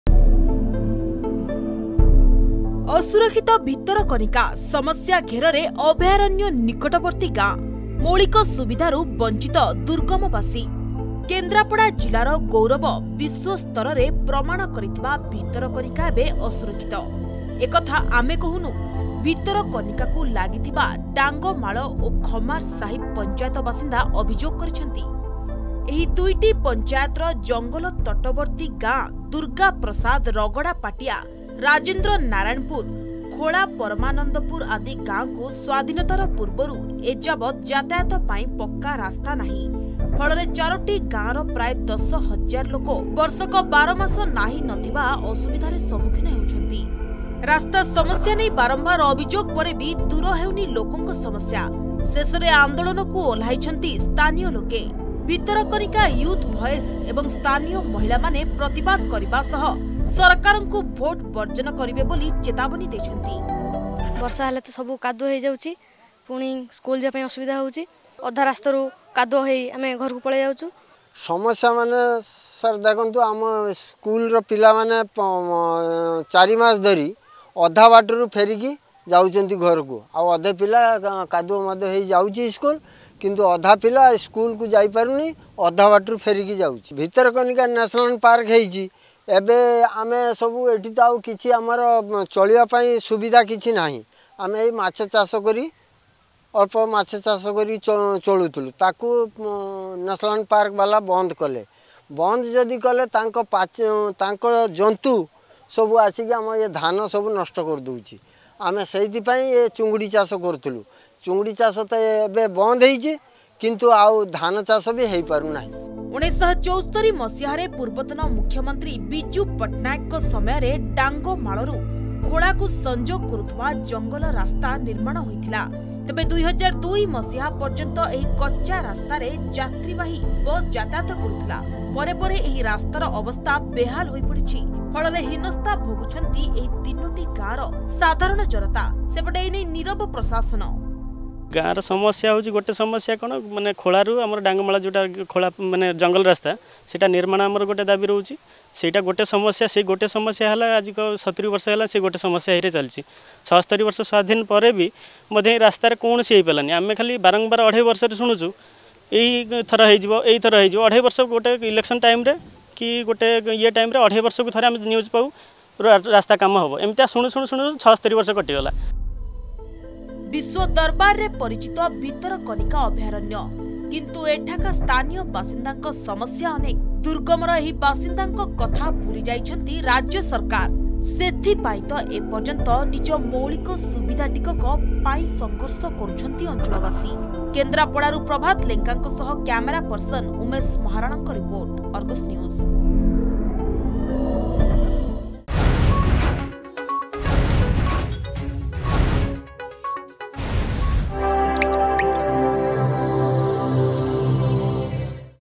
ଅସୁରକ୍ଷିତ ଭିତରକନିକା
ସମସ୍ୟା ଘେରରେ ଅଭୟାରଣ୍ୟ ନିକଟବର୍ତ୍ତୀ ଗାଁ
ମୌଳିକ ସୁବିଧାରୁ ବଞ୍ଚିତ ଦୁର୍ଗମବାସୀ
କେନ୍ଦ୍ରାପଡ଼ା ଜିଲ୍ଲାର ଗୌରବ ବିଶ୍ୱ ସ୍ତରରେ ପ୍ରମାଣ କରିଥିବା ଭିତରକନିକା ଏବେ ଅସୁରକ୍ଷିତ । ଏକଥା ଆମେ କହୁନୁ ଭିତରକନିକାକୁ ଲାଗିଥିବା ଡାଙ୍ଗମାଳ ଓ ଖମାରସାହି ପଂଚାୟତ ବାସିନ୍ଦା ଅଭିଯୋଗ କରିଛନ୍ତି ।